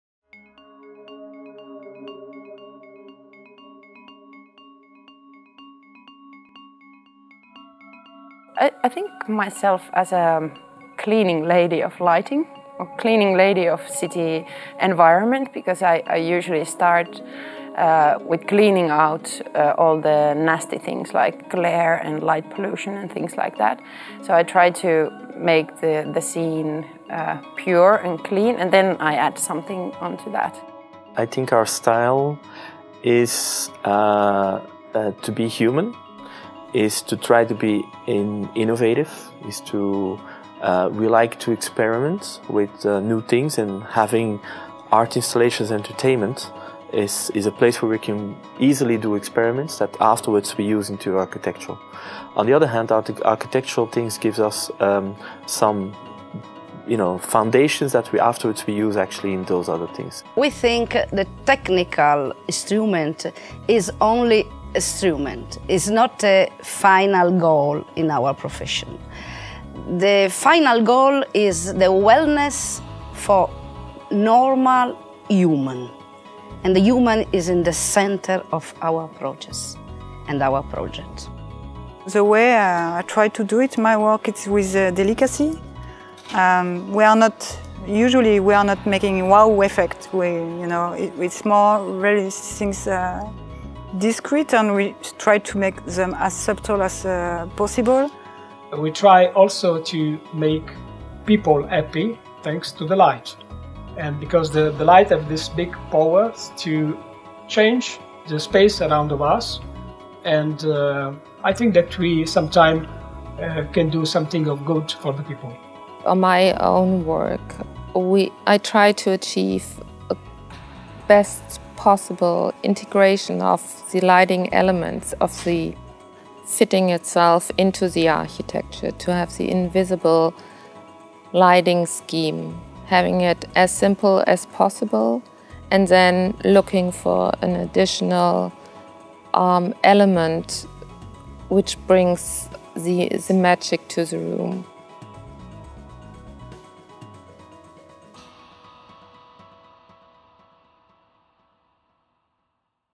Lighting Design in Architecture -- Approaches (Brought to you by Philips Lighting)
Interviews with renowned lighting designers Francesco Iannone & Serena Tellini, Annukka Larsen, Koert Vermeulen, and Carla Wilkins.

At the Professional Lighting Design Convention (PLDC) 2013 in Copenhagen, Philips Lighting spoke to recognized lighting designers.

These discussions represent a continuation of the "Light & Emotions, Exploring Lighting Cultures" book published in 2011 by Birkhäuser, which featured 47 conversations with lighting designers and followed the overwhelming response from designers, architects, engineers, students to the first edition two years earlier.

Lighting Design in Architecture is a 5-part series sponsored by Philips Lighting featuring lighting designers discussing topics based on the Light & Emotions research.

Our thanks go to the beautiful Bella Sky Comwell hotel, designed by 3XN architects, which hosted these conversations.

In this segment, hear from leading lighting designers:
• Annukka Larsen, ALD Larsen, Finland
• Serena Tellini & Francesco Iannone, Consuline, Italy
• Koert Vermeulen, ACT Lighting Design, Belgium
• Carla Wilkins, Lichtvision Design & Engineering, Germany